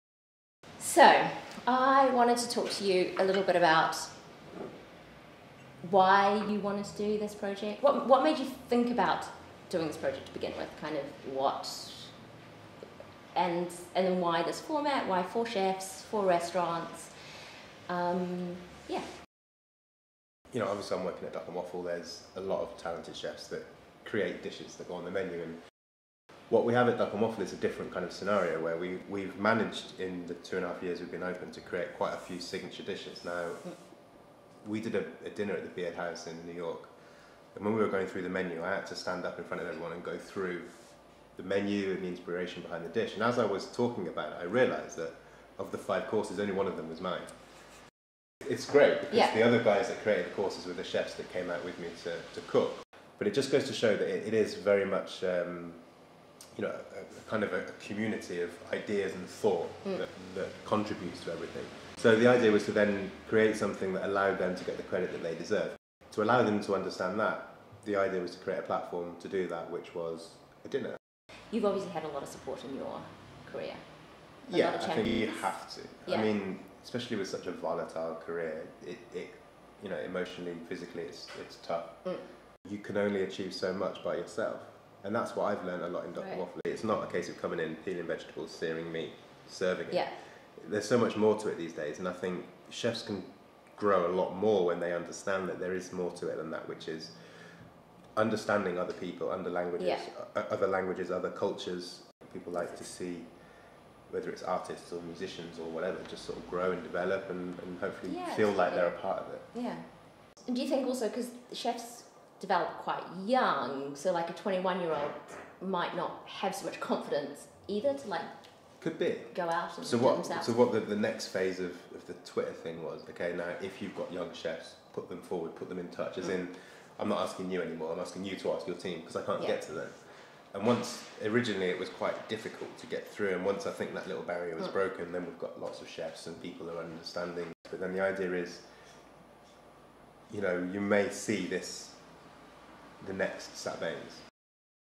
Dan Doherty in conversation with: Anna, Chefs of Tomorrow's producer
In an on-going series, Dan speaks to various people about the project - from restaurateurs, chefs and guests.

In this interview Anna, Chefs of Tomorrow's producer, interviews Dan about his career, those that have supported and inspired him and why he came up with the concept of Chefs of Tomorrow.

Filmed and edited by Ming Tang-Evans
Produced by Anna Sulan Masing

Shot at, and thank you to - Tina, We Salute You in Dalston